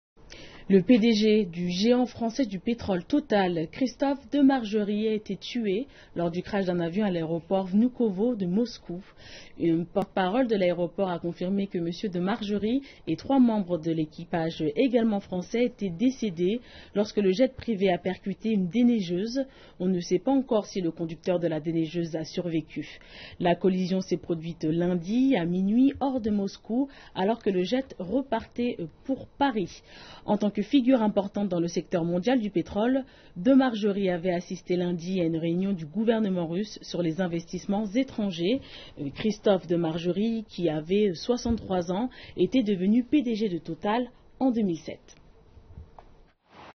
Le PDG du géant français du pétrole Total décédé dans un accident d'avion
Christophe de Margerie, Président-direct général de l'entreprise pétrolière française Total, est décédé lors d'un accident d'avion à l'aéroport de Vnoukovo, à Moscou, dans la nuit de lundi (heure locale). Des enquêtes sont en cours, mais il semblerait le jet où se trouvait l'homme d'affaires ait percuté une déneigeuse sur la piste de décollage, tuant également les trois membres de l'équipage à bord de l'appareil. L'aviation civile russe accuse l'aéroport de négligences graves, tandis que la presse internationale pointe également la responsabilité du conducteur de la déneigeuse, qui aurait été en état d'ivresse.